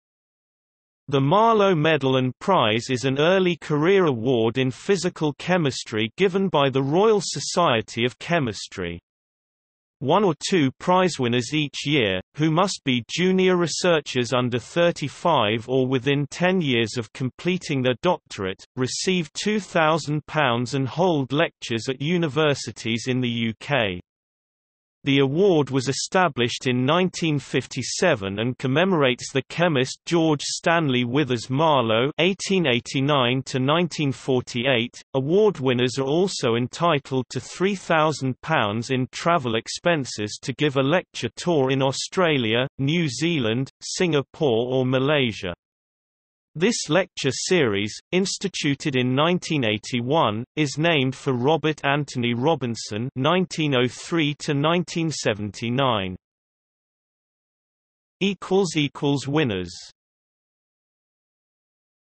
Marlow Award | Wikipedia audio article
- Socrates

SUMMARY
The Marlow Medal and Prize is an early-career award in physical chemistry given by the Royal Society of Chemistry. One or two prizewinners each year, who must be junior researchers under 35 or within 10 years of completing their doctorate, receive £2000 and hold lectures at universities in the UK. The award was established in 1957 and commemorates the chemist George Stanley Withers Marlow (1889–1948).Award winners are also entitled to £3000 in travel expenses to give a lecture tour in Australia, New Zealand, Singapore or Malaysia. This lecture series, instituted in 1981, is named for Robert Anthony Robinson (1903–1979).